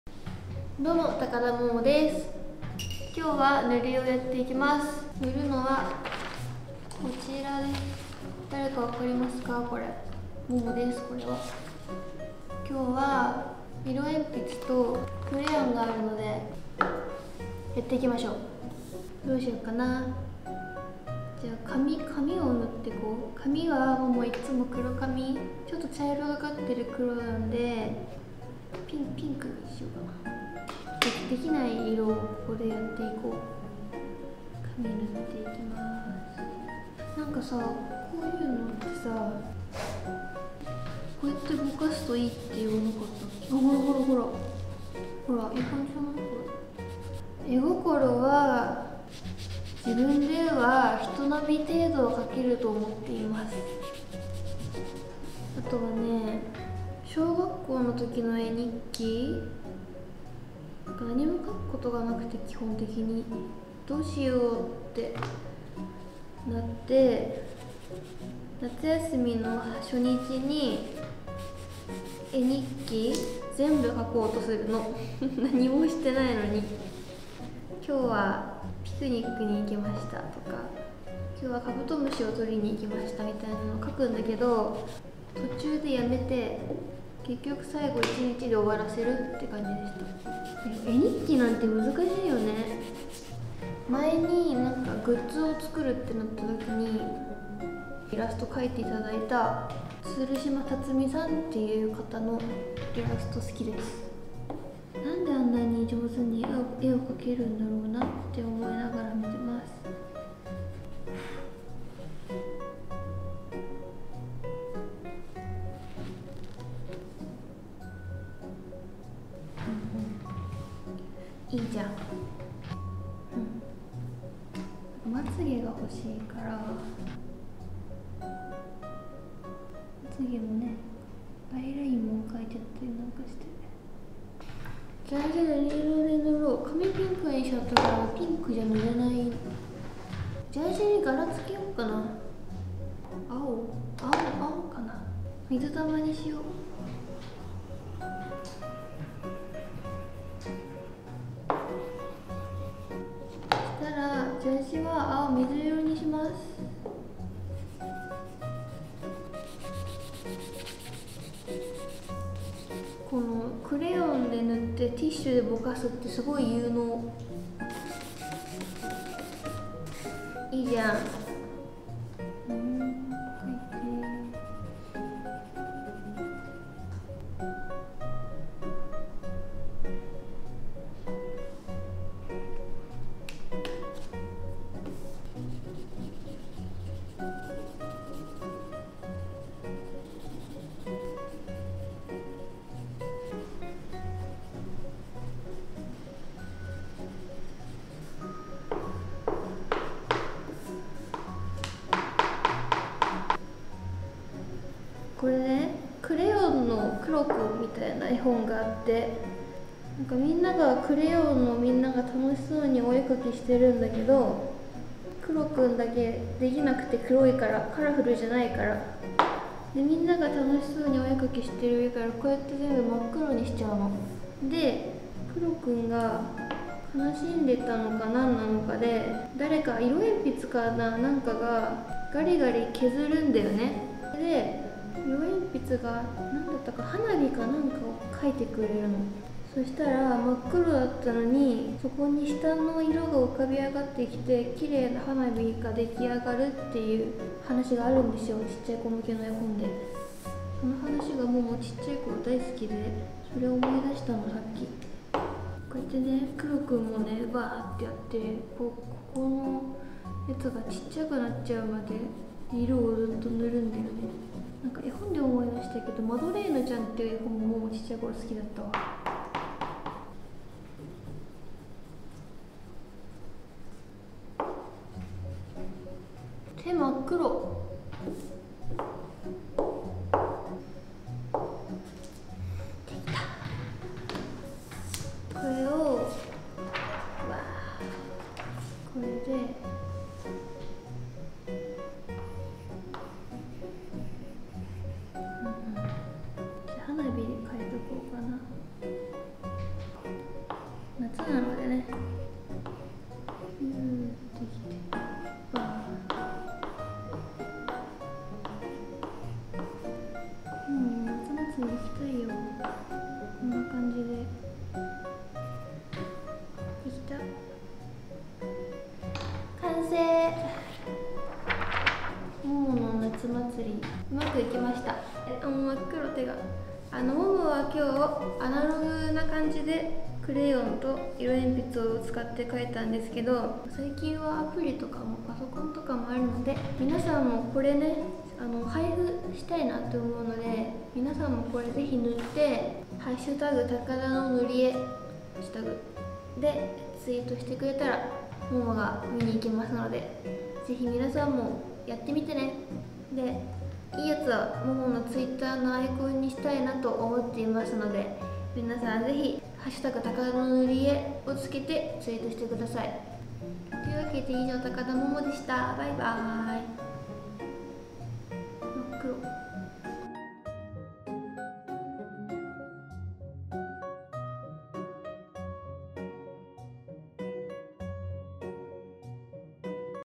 ぬりえをしよう。
高田もも生誕記念スペシャルウィーク第１弾！！

今回は塗り絵をしてみました。
久しぶりクレヨンを使ったり、楽しかったし
それなりに上手くできたんじゃないかなと思います。

みんなにも遊んでもらえるように今回使った塗り絵を配布してます！

塗ったら、ぜひ 高田の塗り絵 でTwitterやInstagramにアップしてください。
高田がエゴサして観にいきます。
すごいのがあったら、SNSのアイコンに使わせていただくかもです。。。

みんなの作品、たのしみにしてます！